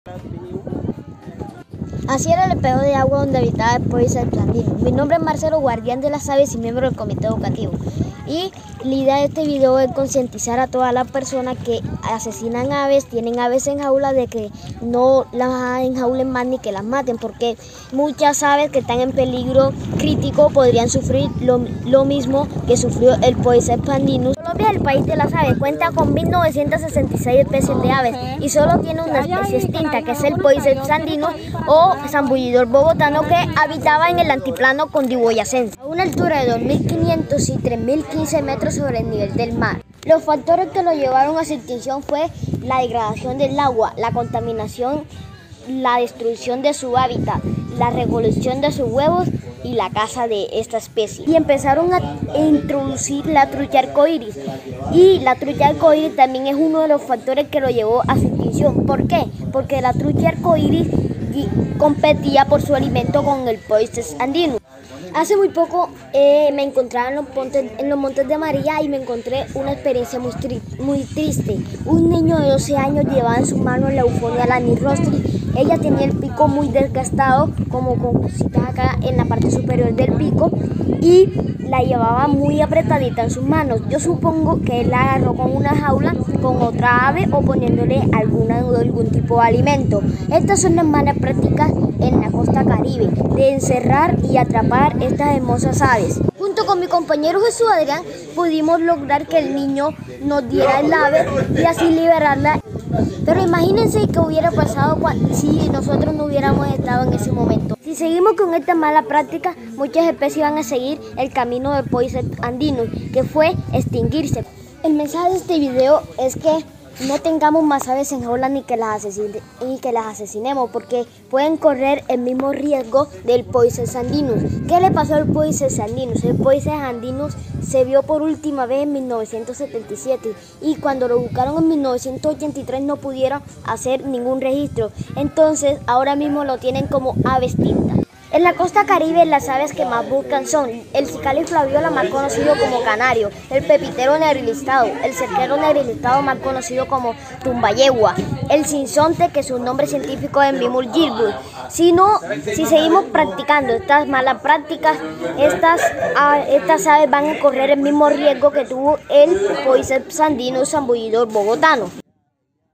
Marcelo Argel desde Sucre
Nos habla sobre la Única especie de ave extinta que tiene Colombia, El Podices andinus